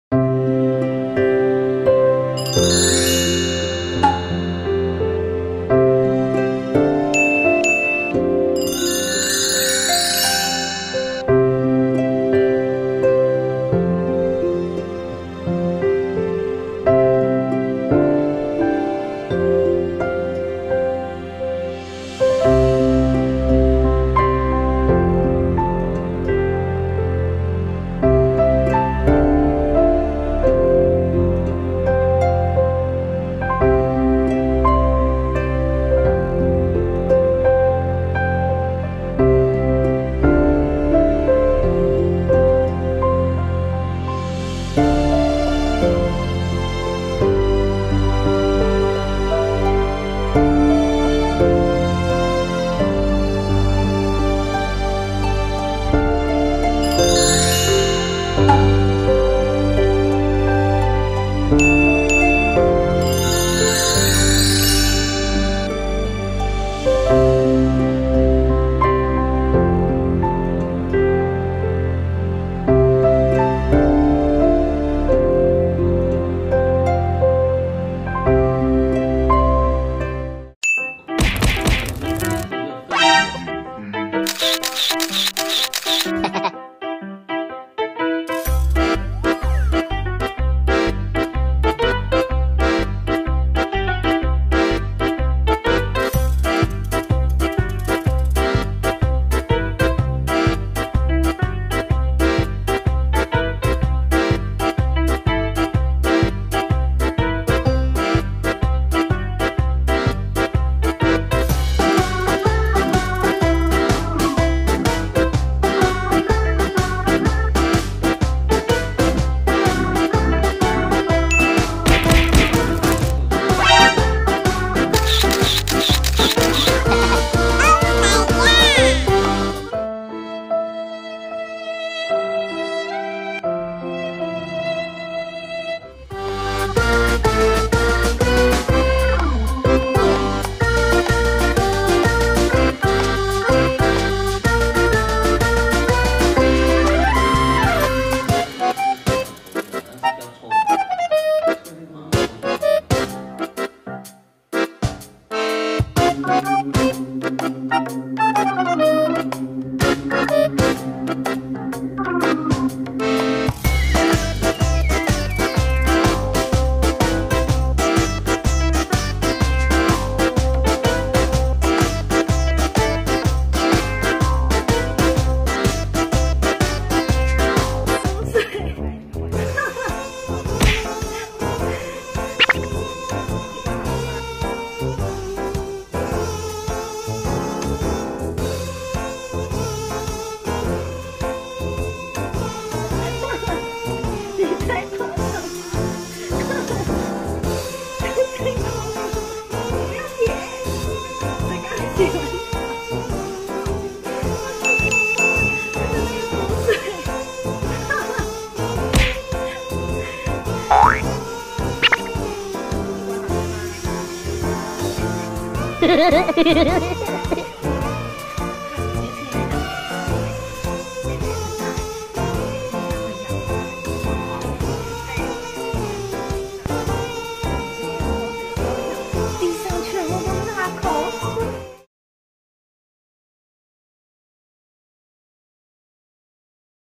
暖爸抱法鬥犬溫柔拍背 「小瘦子」桶裝身材壓著如胸口碎大石｜寵物｜法鬥｜討抱抱
爸爸一回家，圓嘟嘟法鬥就趴在他身上討抱抱，過著甜蜜的兩人世界(*´∀`)~♥
推薦觀看...

☑影片授權：網友 沈涵  授權